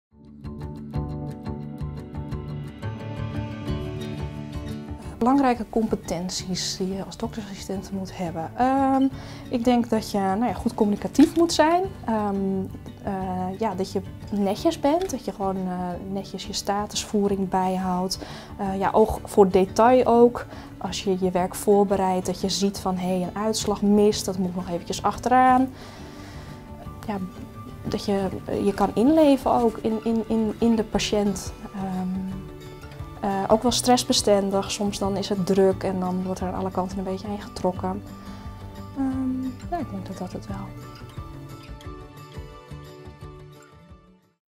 Doktersassistent - Wat maakt een doktersassistent goed?
Waaraan voldoet een goede doktersassistent? Moet je goed met mensen kunnen omgaan of bijvoorbeeld heel punctueel zijn?